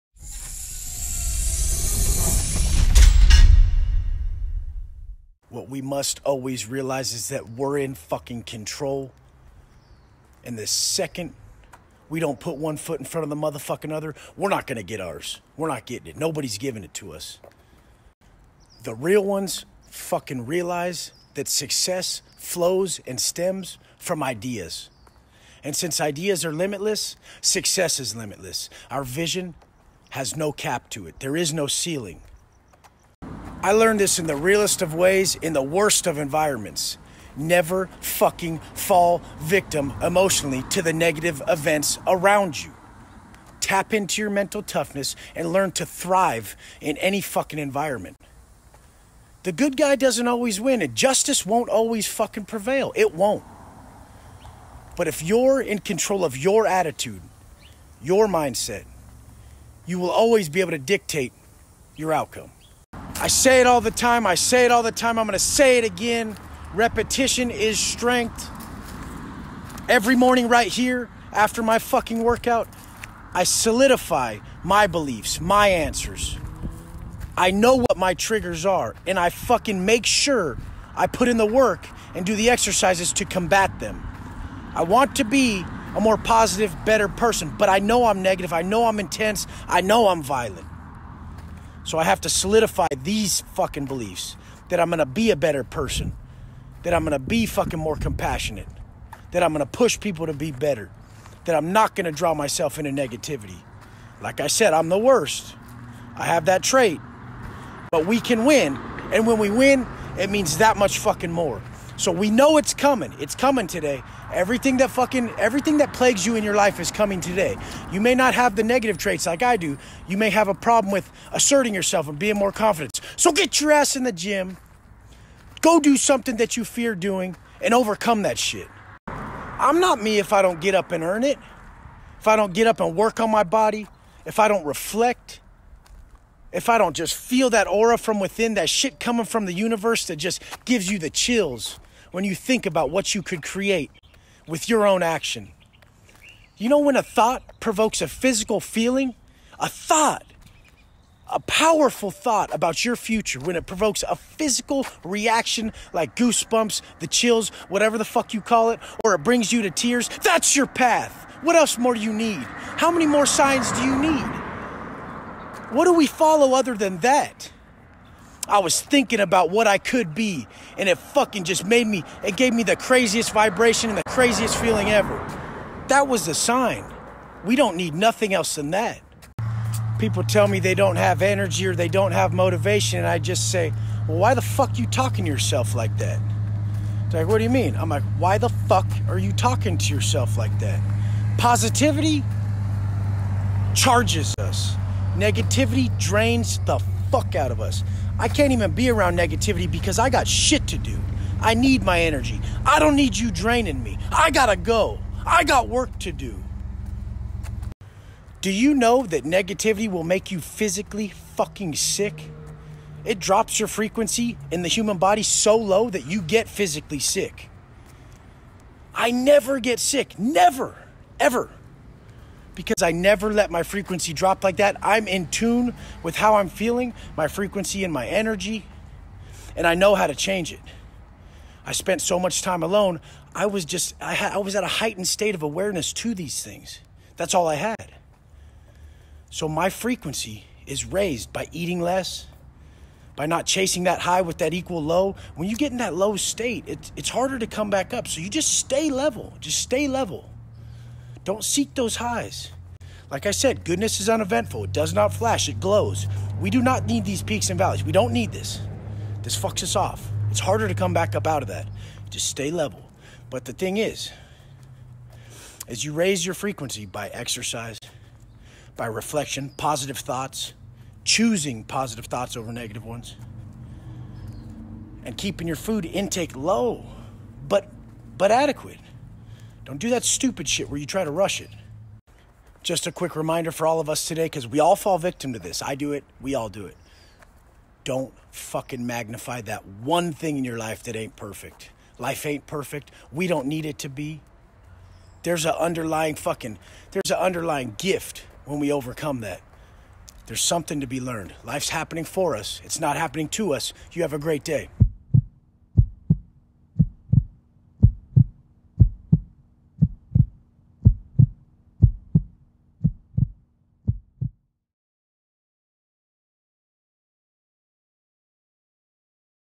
Wes Watson- Walkin The Yard: The Real Ones
The real ones realize that success flows and stems from ideas... In this episode, Wes Watson shares that since ideas are limitless, success is limitless and our vision has no cap to it